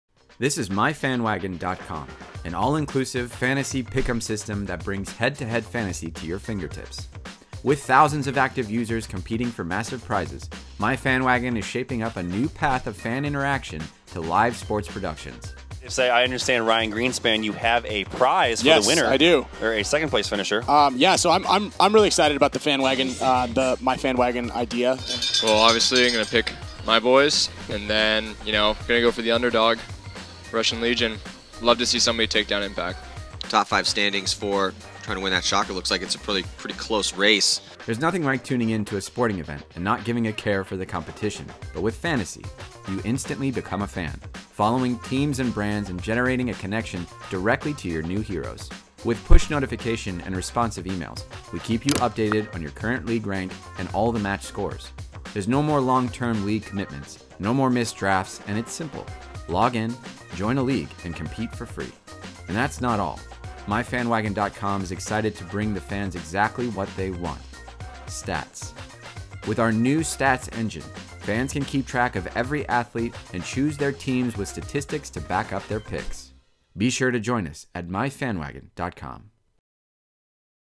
Fantasy Paintball and PAINTBALL STATS!
Paintball stats are back!

Tune in to the GOSPORTS.COM Webcast to see the new Myfanwagon Stats engine powering all the NXL paintball Stats! Kill count, Points played, KP Ratio and more! Join the fans and play Fantasy Paintball, or sit back and watch the kills rack up!

Prizes so far include the new DLX Shocker, Empire Vanquish, GI Sportz Grillz and so much more to come!

Want to help support fantasy and stats? - Hit us up! We would love to talk!